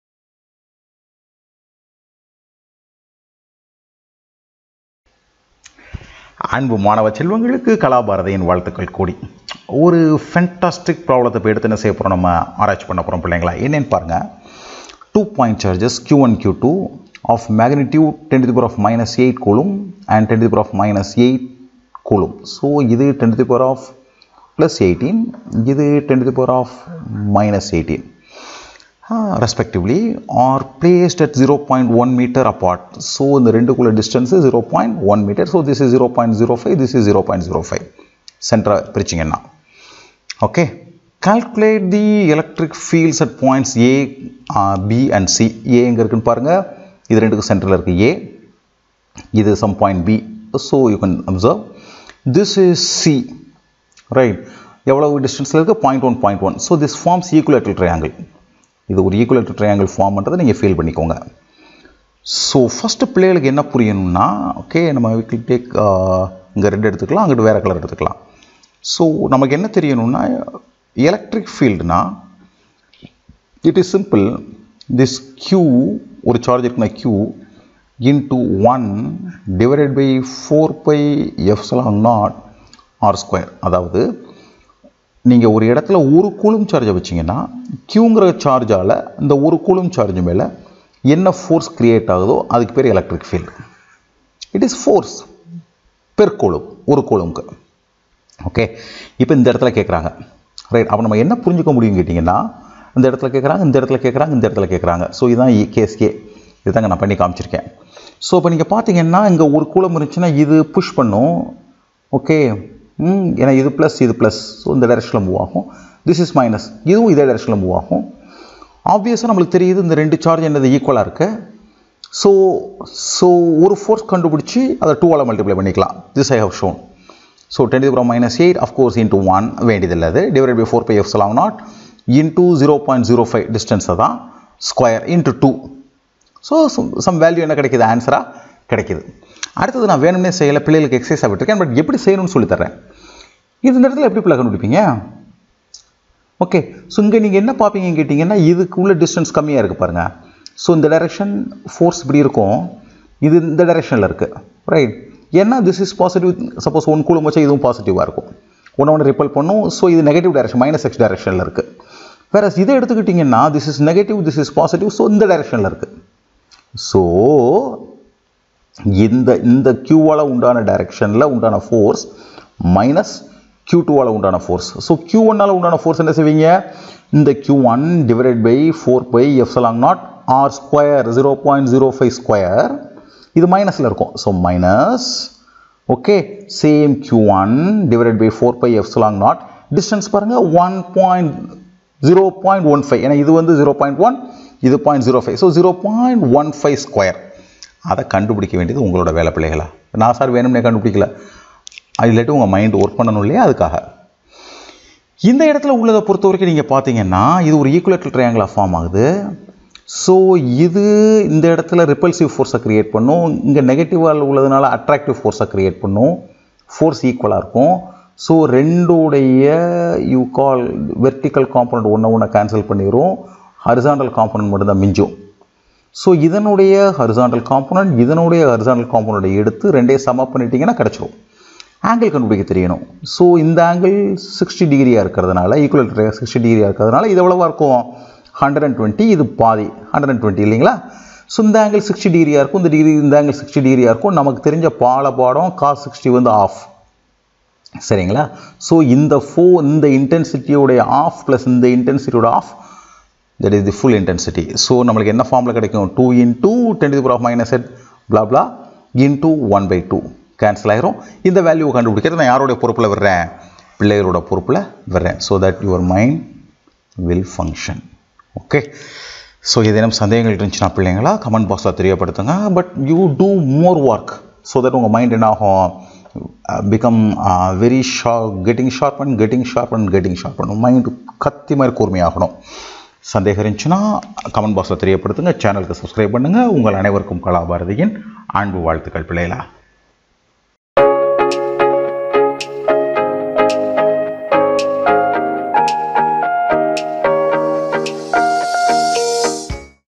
9.Physics | Electric Charges and Fields | Example 1.9
Two point charges q1 and q2, of magnitude +10–8 C and –10–8 C, respectively, are placed 0.1 m apart. Calculate the electric fields at points A, B and C shown in Fig. 1.14.